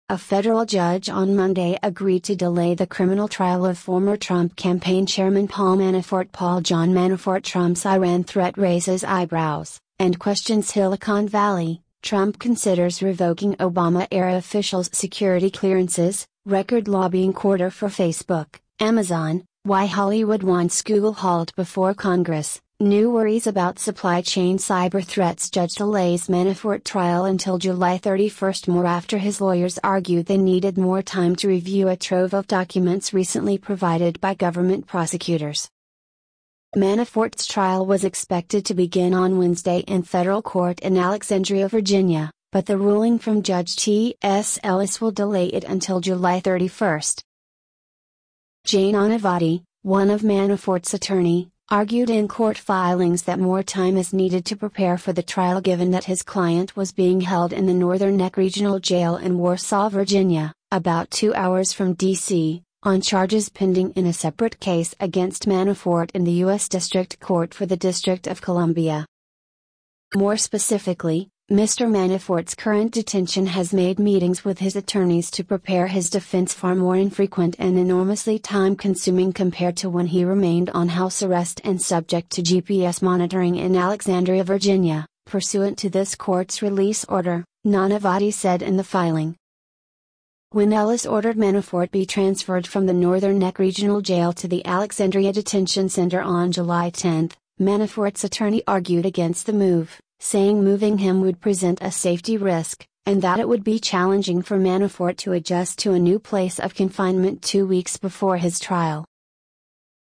Judge delays Manafort trial until July 31
A federal judge on Monday agreed to delay the criminal trial of former Trump campaign chairman Paul Manafort after his lawyers argued they needed more time to review a trove of documents recently provided by government prosecutors.